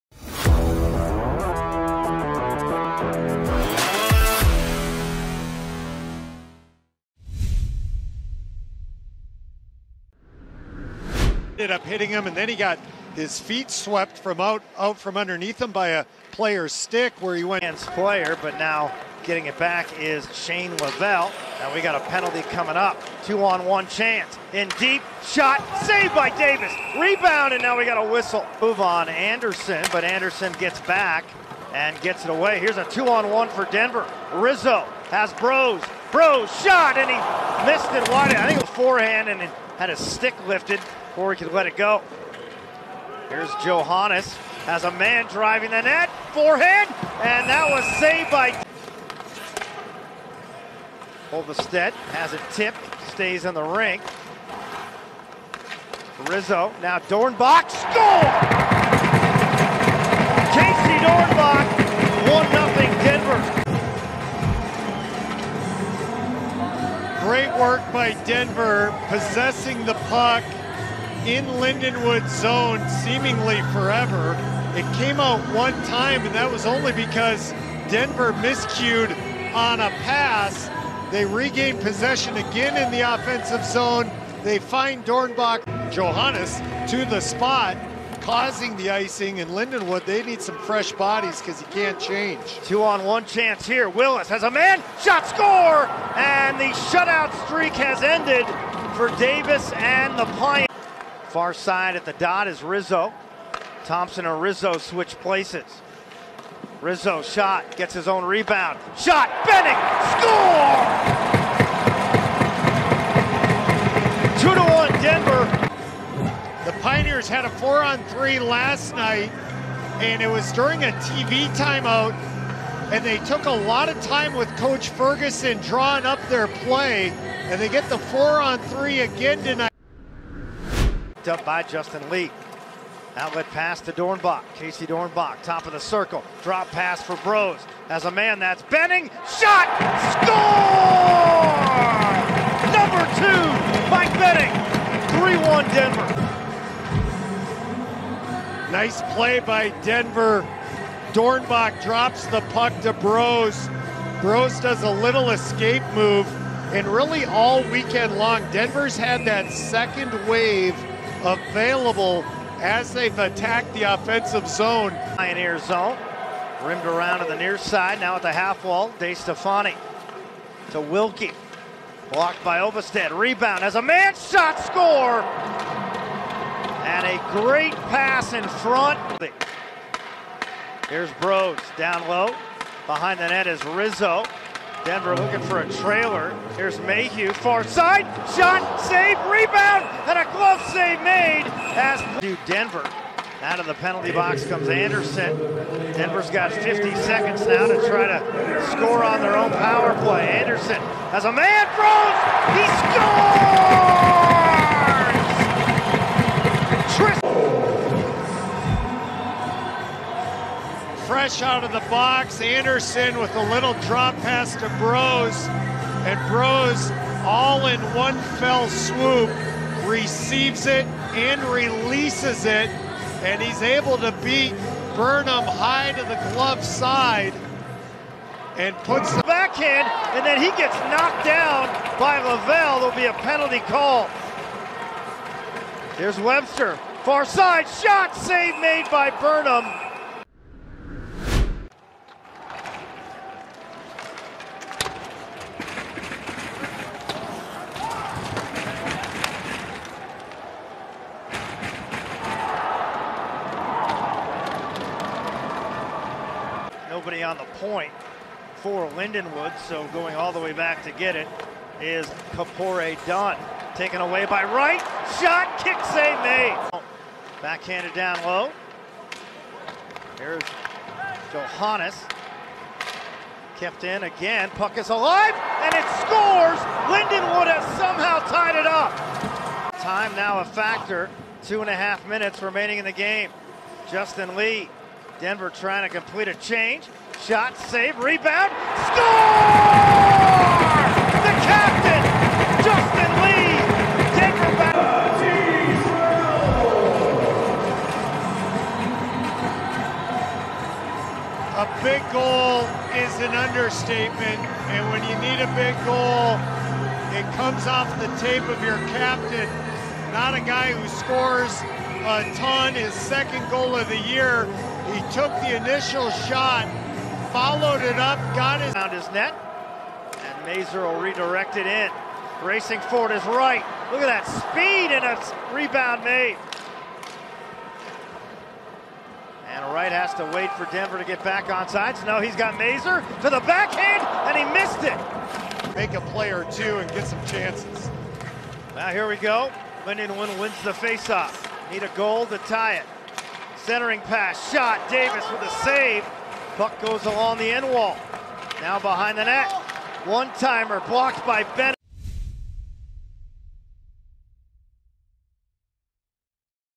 12/17/22 - Lindenwood at Denver Highlights
Highlights of DU's 5-4 win over Lindenwood on Saturday, Dec. 17, 2022 at Magness Arena in Denver, Colo.